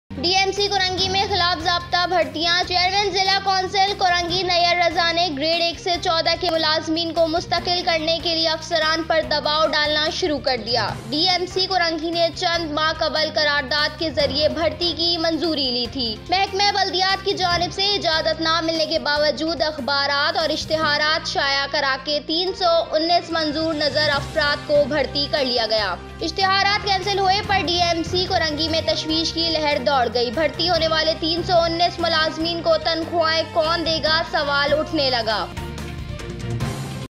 Recruitment against DMC Korangi
ڈی ایم سی کورنگی میں خلاف ضابطہ بھرتیاں ۔۔چیئرمین  ضلع کونسل کورنگی نیئررضانے گریڈ ایک سے چودہ کے ملازمین کومستقل کرنے کے لیے افسران پردباوڈالناشروع کردیاڈی ایم سی کورنگی نے چندماہ قبل قرارداد کے ذریعے بھرتی کی منظوری لی تھی محکمہ بلدیات کی جانب سے اجازت نہ ملنے کے باوجوداخبارات میں اشتہارات شائع کراکے تین سواینس منظورنظراافرادکوبھرتی کرلیاگیااشتہارات کینسل ہونے پرڈی ایم سی کورنگی میں تشویش کی لہردوڑگئی۔بھرتی ہونیوالے تین سوانیس ملازمین کوتنخواہیں کون دیگاسوال اٹھنے لگے